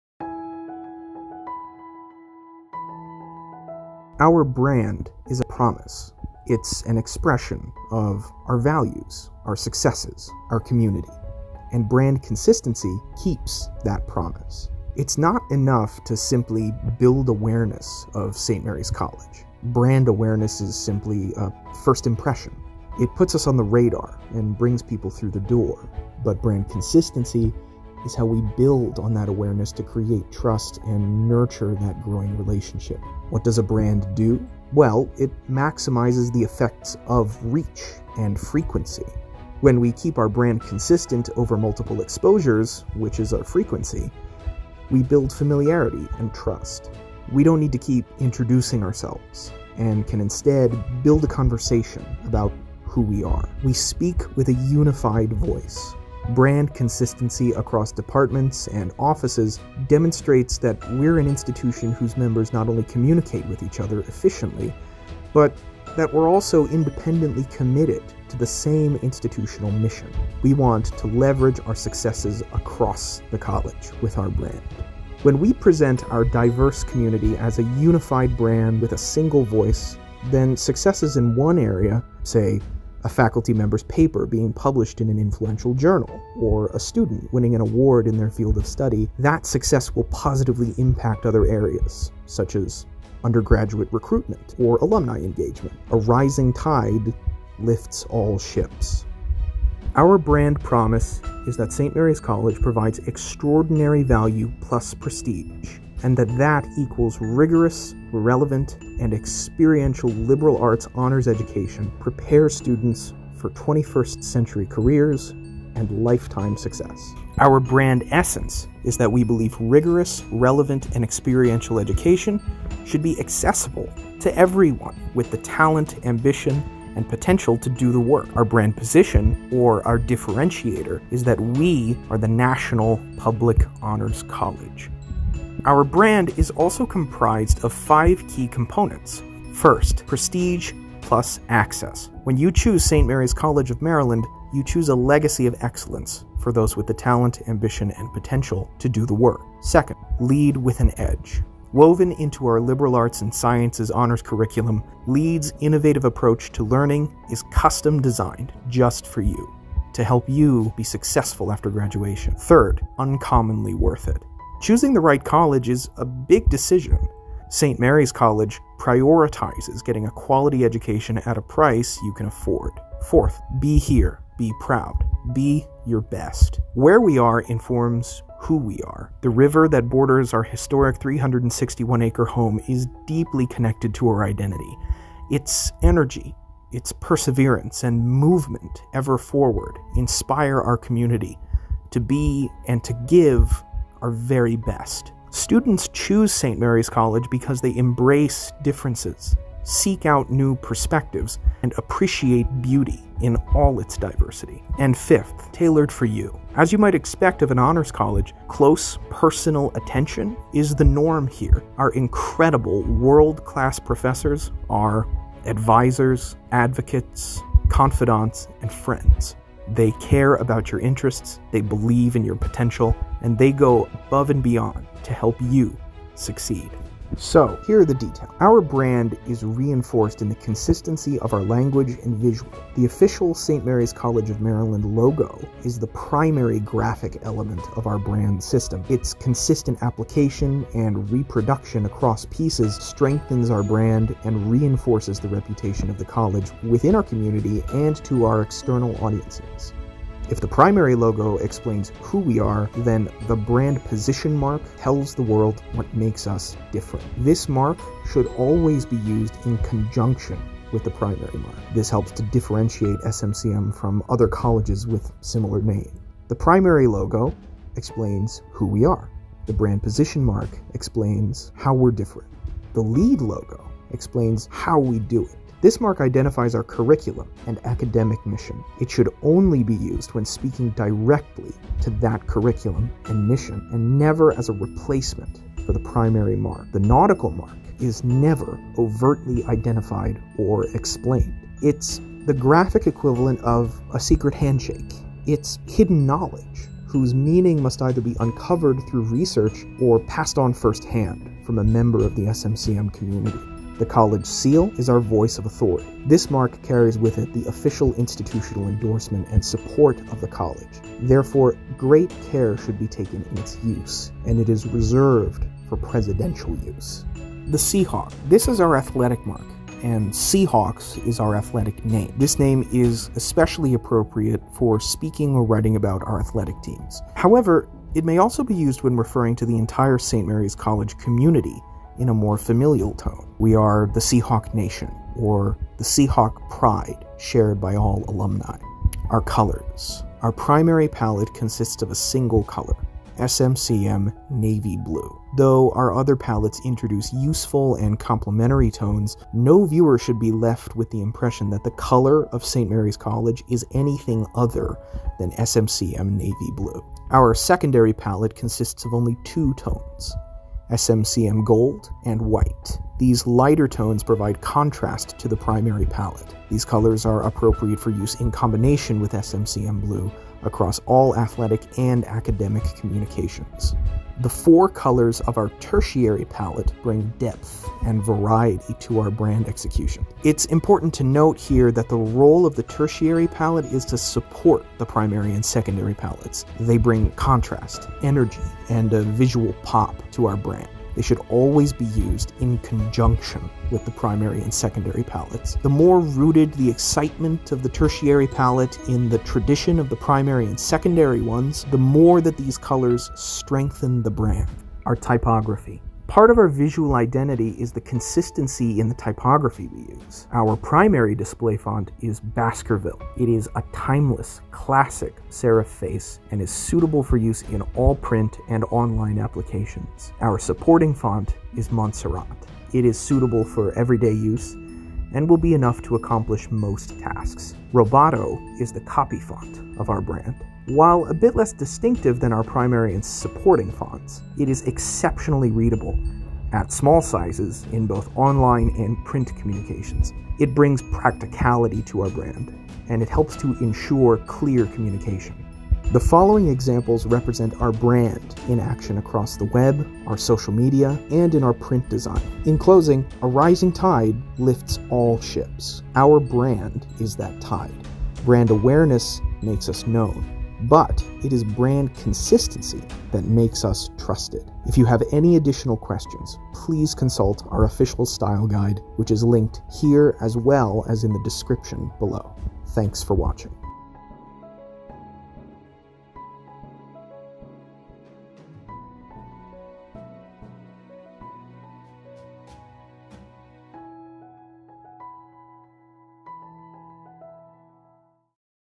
SMCM Brand Training Movie
Overview and rationale of the St. Mary's College of Maryland brand.
Produced by Keely Houk '17 for the Office of Integrated Marketing